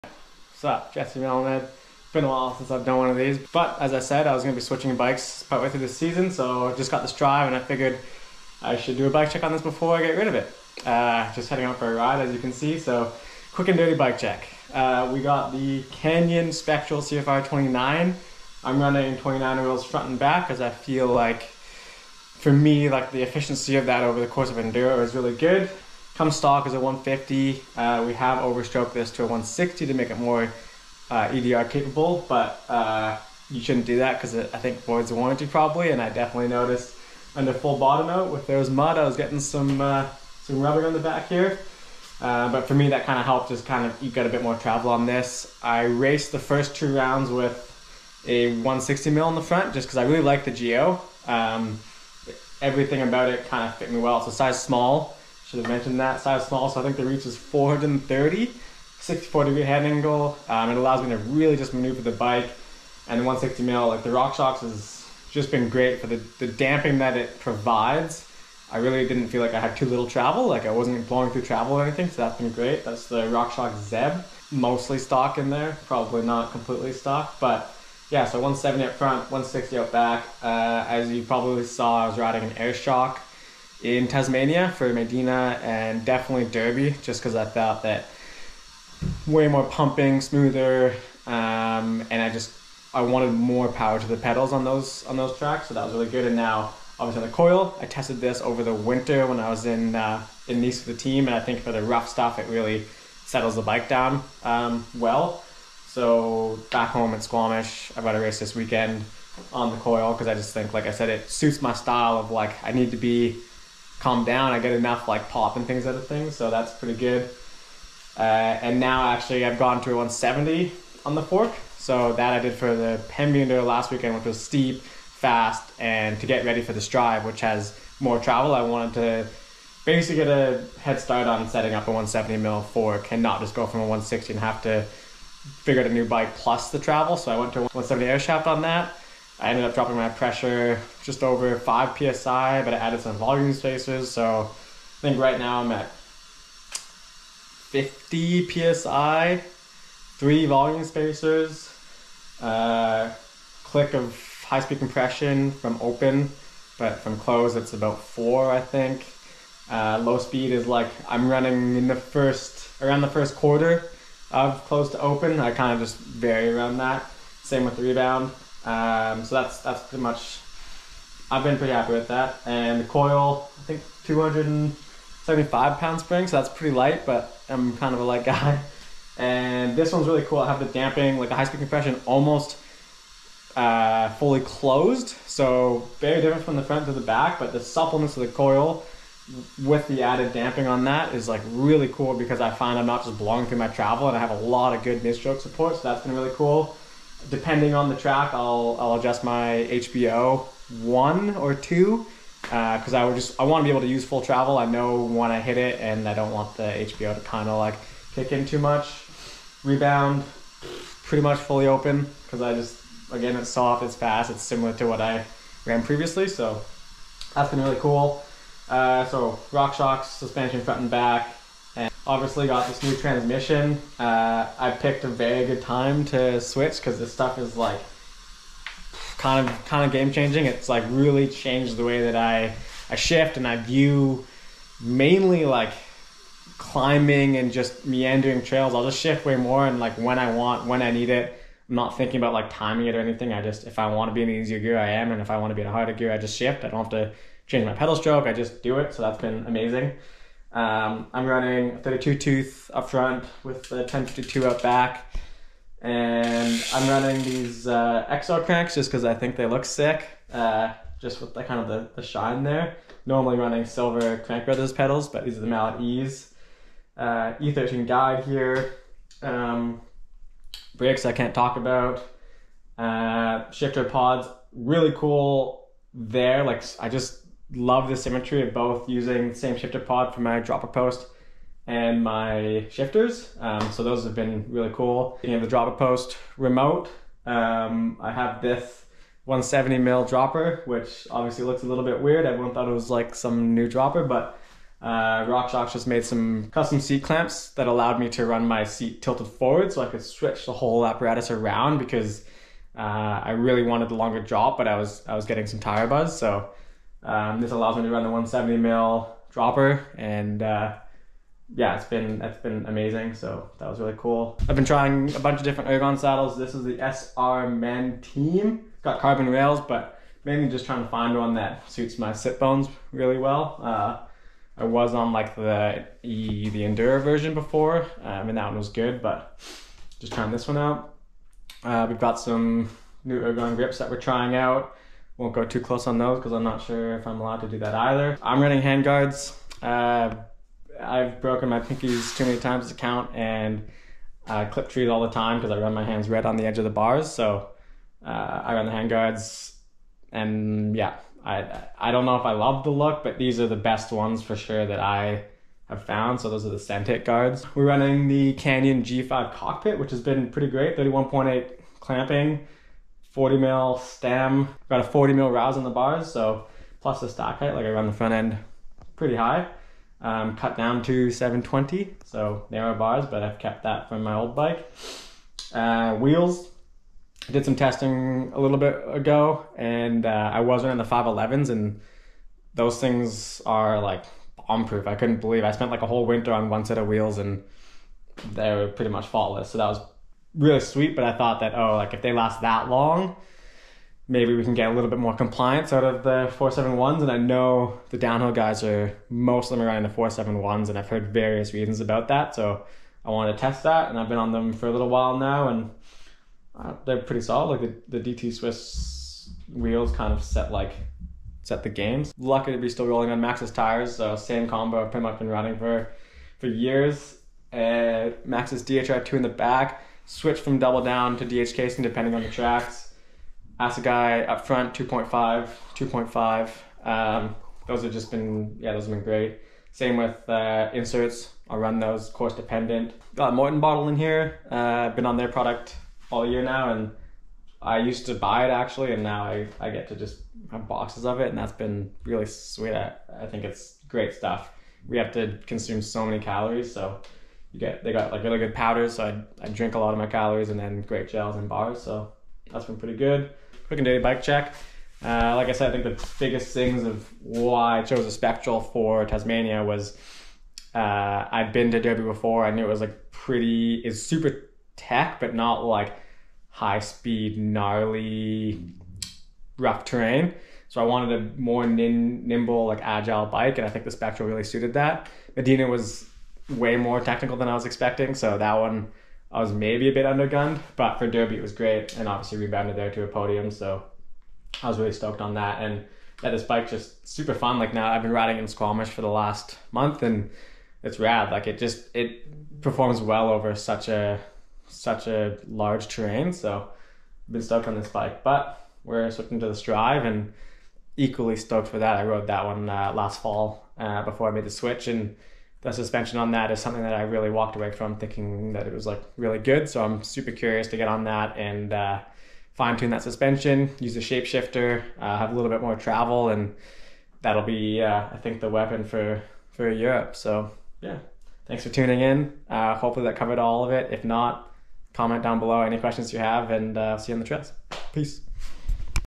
THE WHY - My Canyon Spectral Bike check
29er? 170mm? SMALL!? Find out all the things, and if I missed something just ask below.